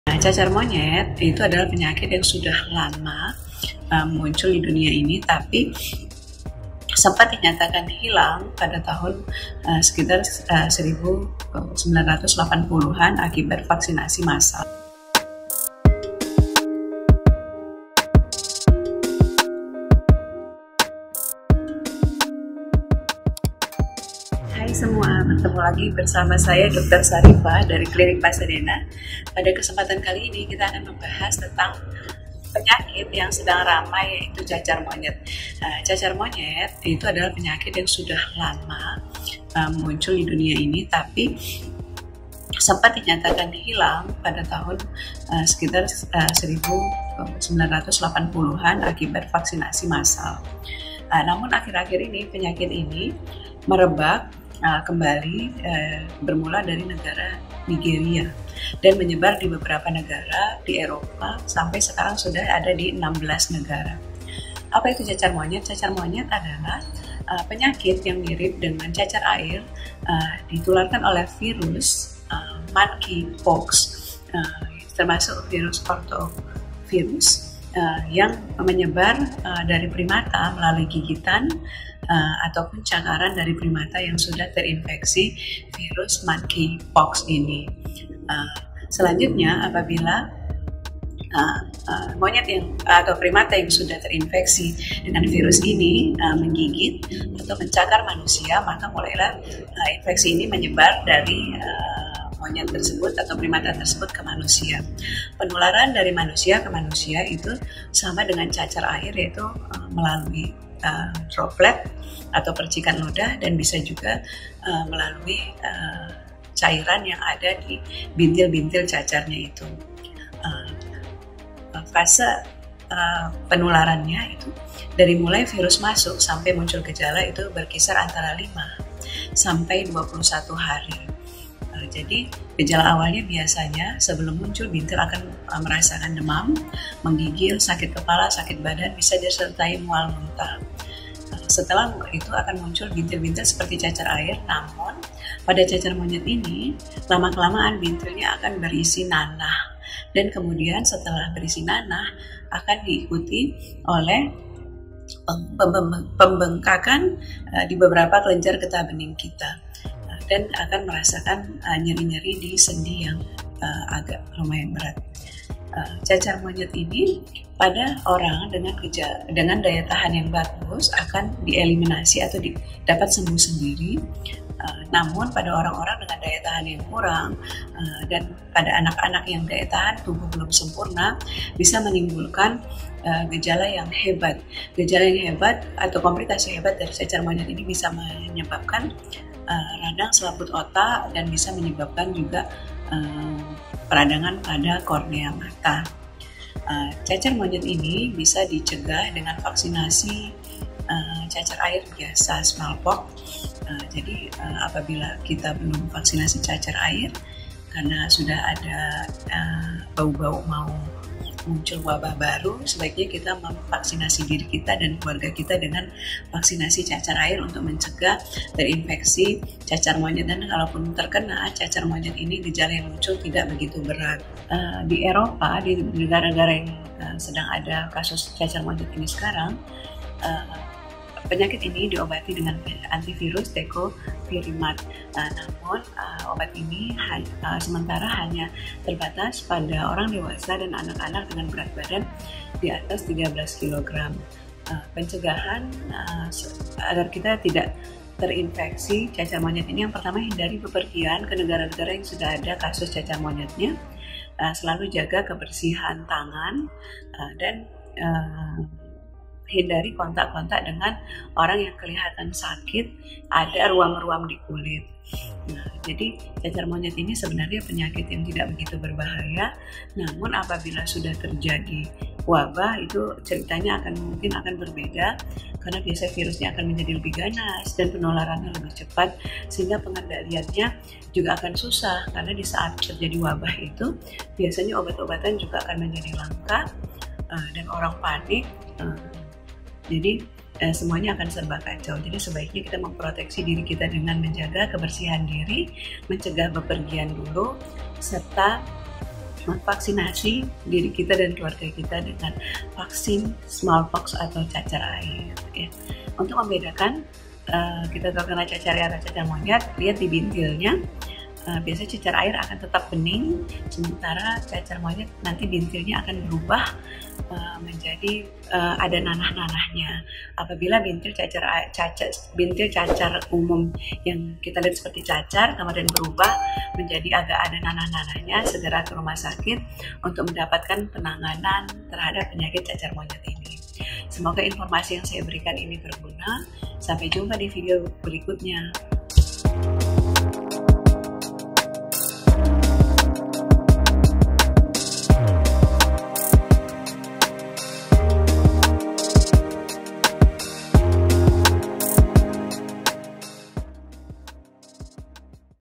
CACAR MONYET (monkeypox ) || VIRUS DENGAN CIRI BINTIL BERNANAH
penyakit cacar monyet memiliki gejala yang serupa dengan cacar air, yaitu bintil berair. Seiring perkembangan penyakit, bintil berair berubah menjadi bernanah dan menimbulkan benjolan di leher, ketiak, atau selangkangan akibat pembengkakan kelenjar getah bening.
Cacar monyet merupakan penyakit yang dapat menular dari orang ke orang, tetapi sumber utamanya adalah hewan pengerat dan primata, seperti tikus, monyet, atau tupai yang terinfeksi.

Penyebab Cacar Monyet
Cacar monyet disebabkan oleh virus monkeypox, yaitu virus yang termasuk dalam kelompok Orthopoxvirus. Virus ini awalnya menular dari hewan ke manusia melalui cakaran atau gigitan hewan, seperti tupai, monyet atau tikus, yang terinfeksi virus monkeypox.

Penularan virus monkeypox juga dapat terjadi lewat kontak langsung dengan cairan tubuh hewan yang terinfeksi.

Cacar monyet menyebar antarmanusia melalui percikan liur yang masuk melalui mata, mulut, hidung, atau luka di kulit. Penularan juga bisa terjadi melalui benda yang terkontaminasi, seperti pakaian penderita. Namun, penularan antarmanusia membutuhkan kontak yang lama.

Untuk informasi lebih lengkap ikut videonya bersama dokter Syarifah Azizah dari klinik Pasadena,